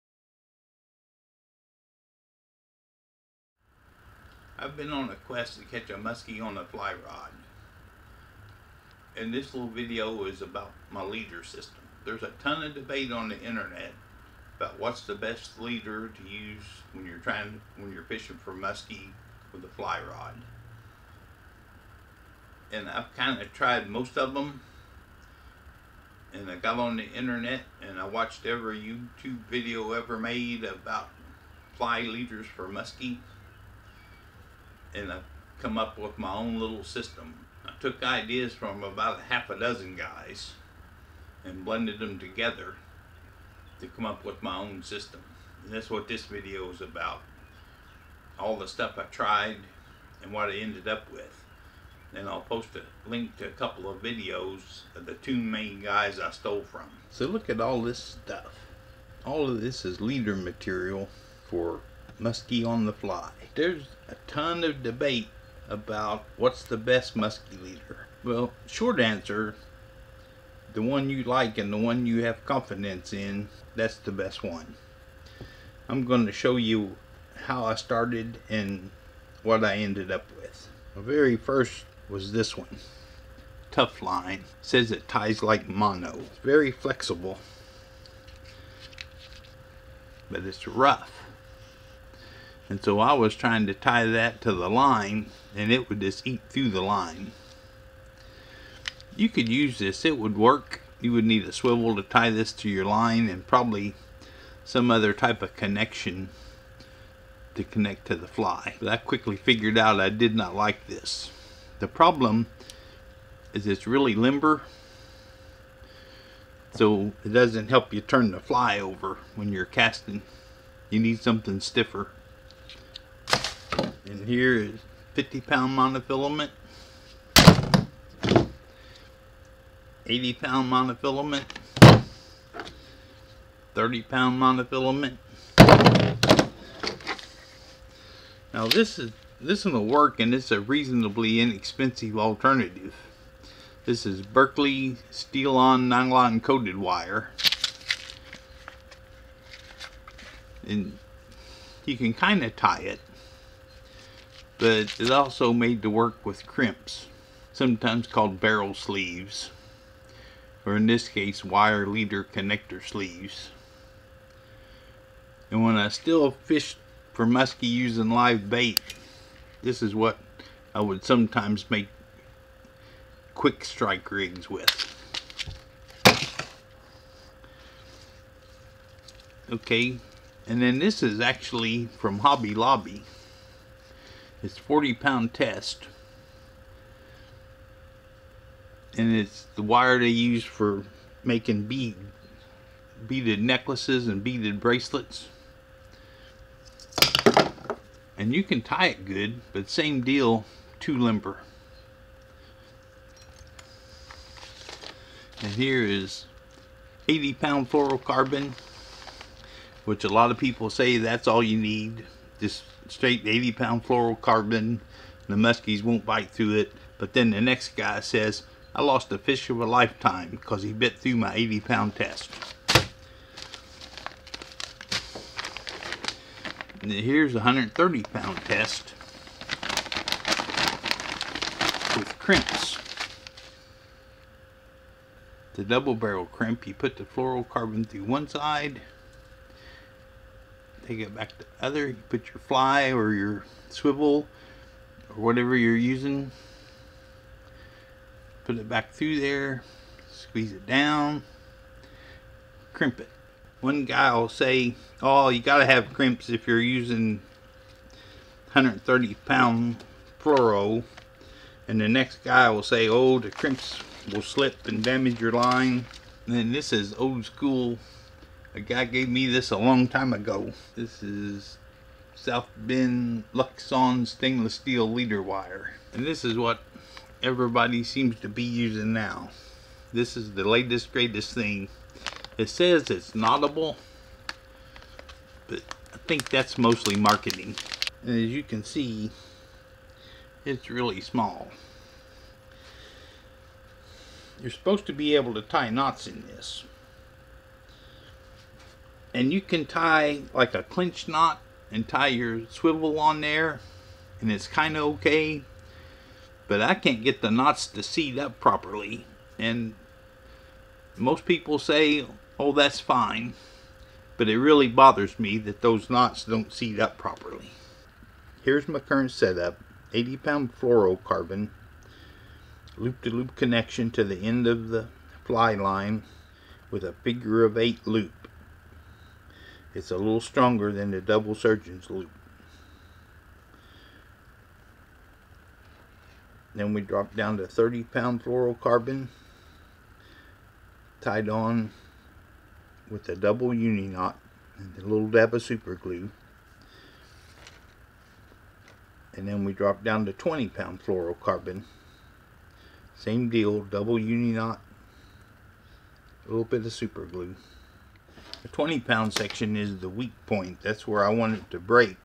Muskie on the Fly, Best Leader! Knot 2 Kinky wire + fluorocarbon
Trying to find the best leader for Muskie fishing with a fly rod?  Here is what I used previously and what I use now.